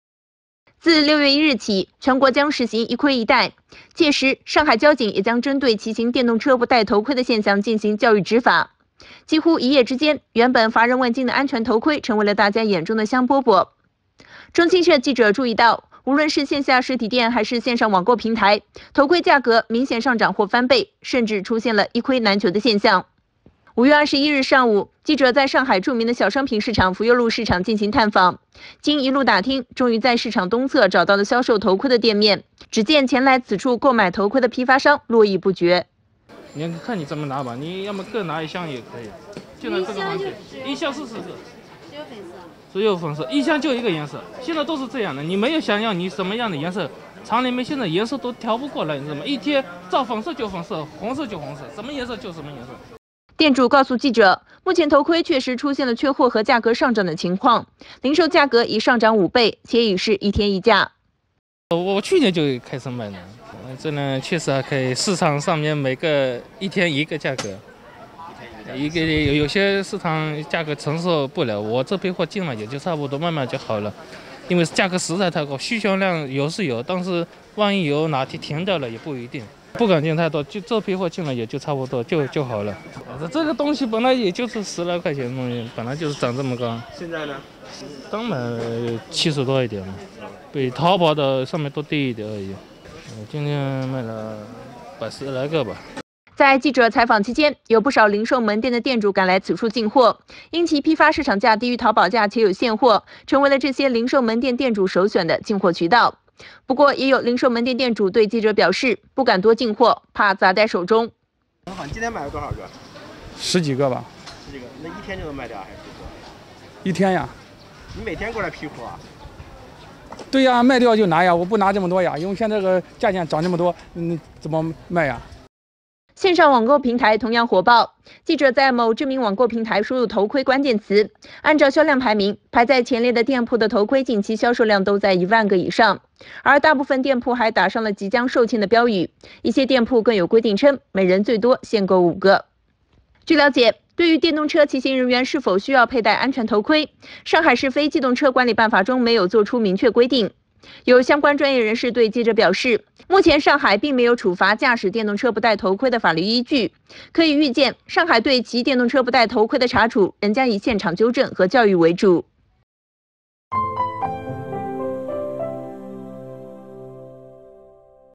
中国6月将实行“一盔一带” 上海头盔市场：零售价已涨5倍
自6月1日起，全国将实行“一盔一带”。届时，上海交警也将针对骑行电动车不戴头盔的现象，进行教育执法。几乎一夜之间，原本乏人问津的安全头盔成为了大家眼中的“香饽饽”。中新社记者注意到，无论是线下实体店还是线上网购平台，头盔价格明显上涨或翻倍，甚至出现了“一盔难求”的现象。